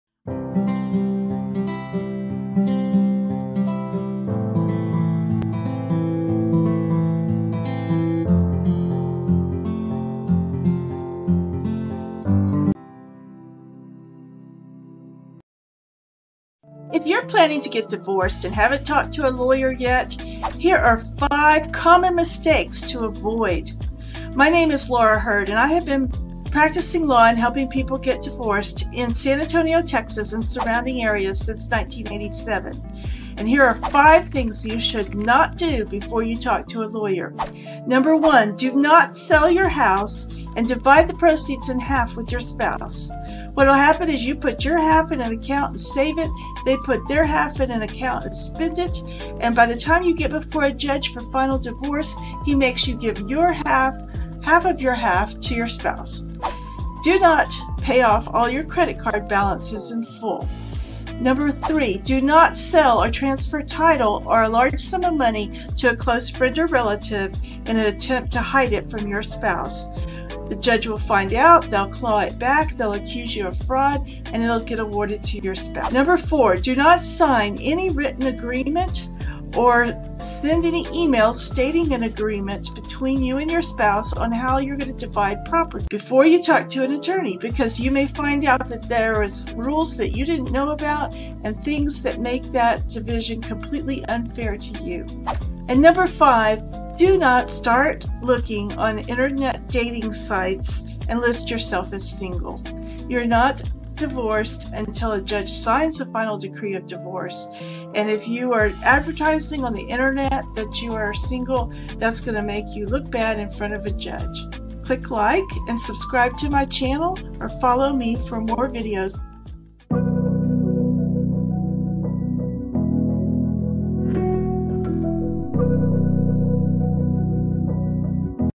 5 COMMON MISTAKES TO AVOID BEFORE FILING A DIVORCE
Here are 5 mistakes people often make when considering divorce. texasdivorce texasfamilylaw texasattorney divorce lauraheardlawfirm.